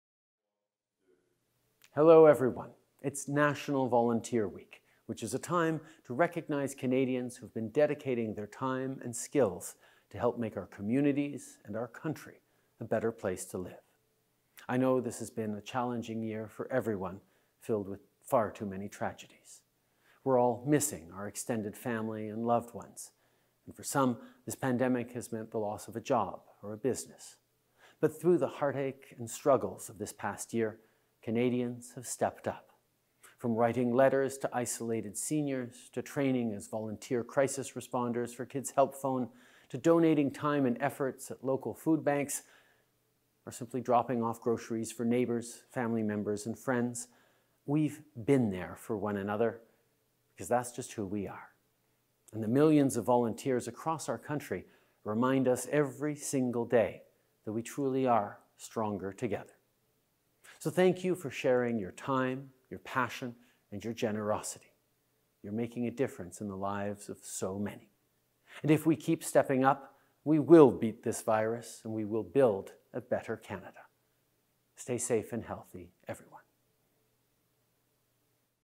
Prime Minister's message on National Volunteer Week
Prime Minister Justin Trudeau pays tribute to volunteers across Canada